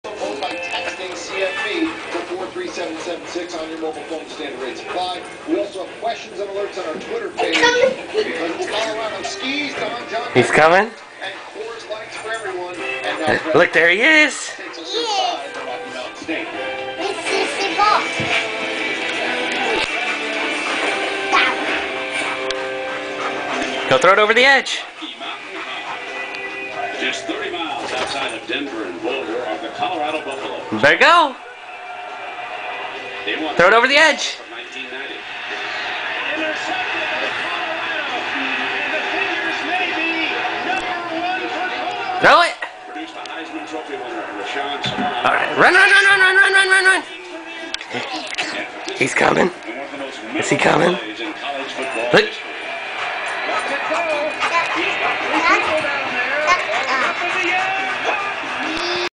Playing with Sampson
Playing fetch with Sampson.